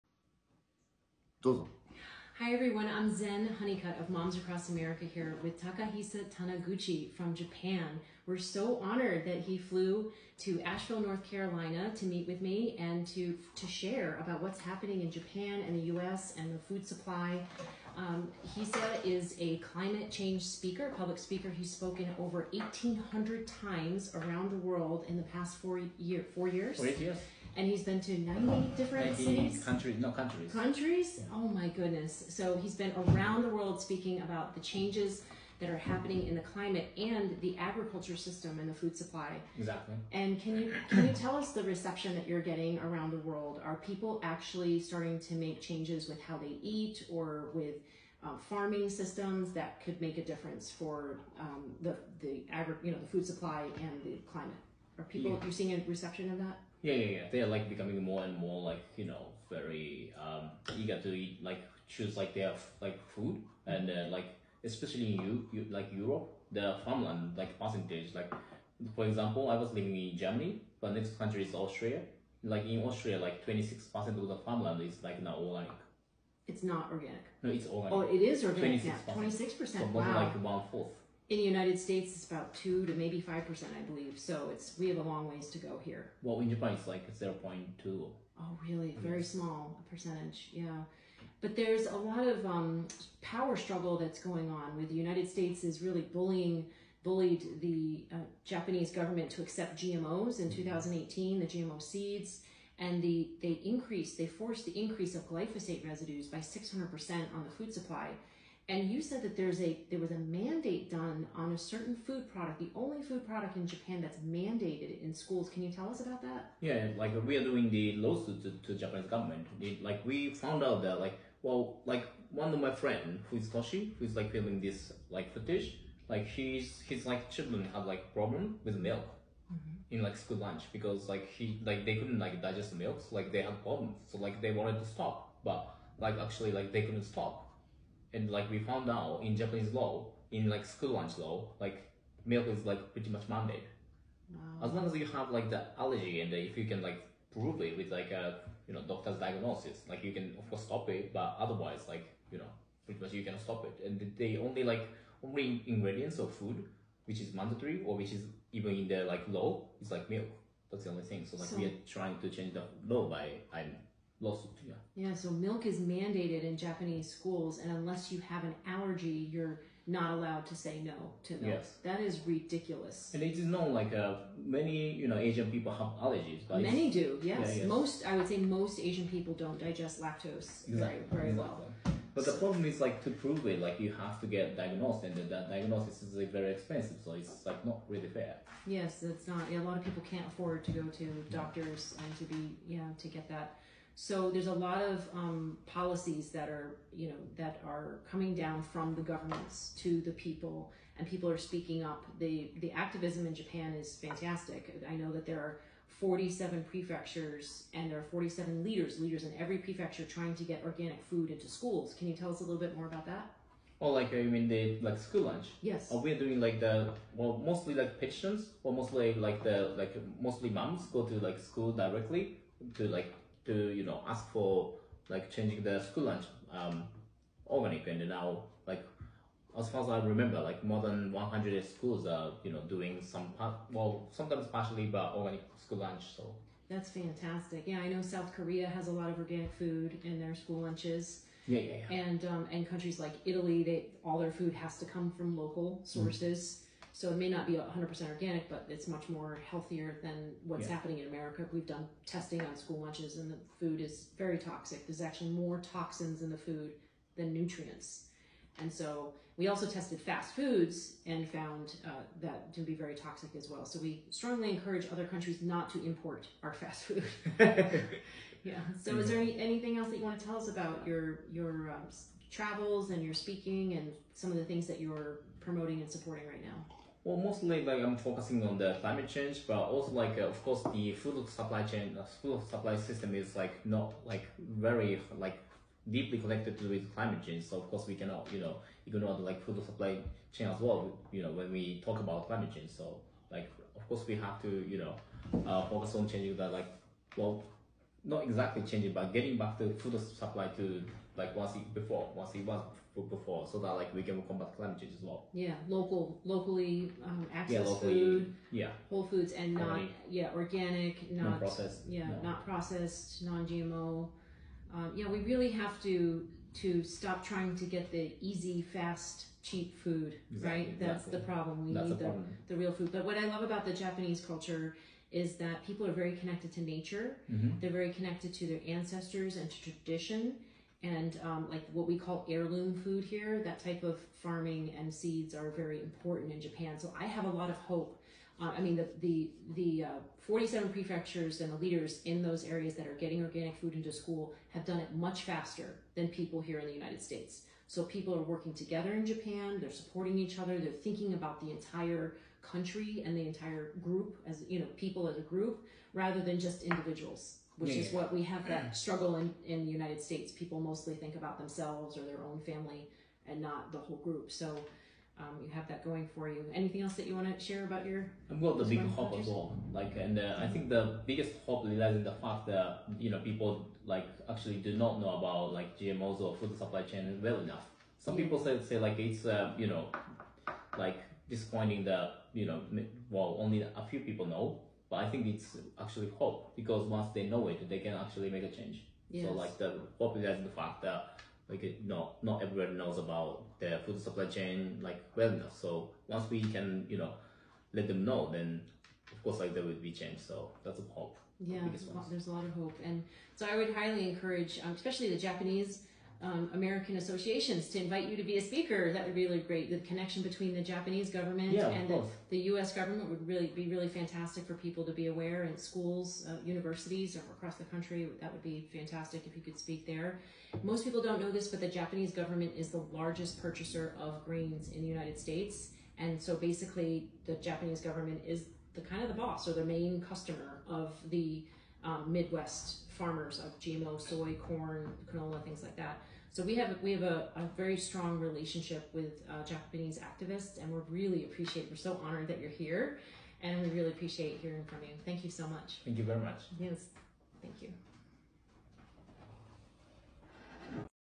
Takahisa Taniguchi talks with Moms Across America about Japanese School Food
Environmental activist, entrepreneur, speaker, and author @takahisa_taniguchi talks with @zenhoneycutt, founder of @momsacrossamerica about what is mandated to consume in Japanese schools.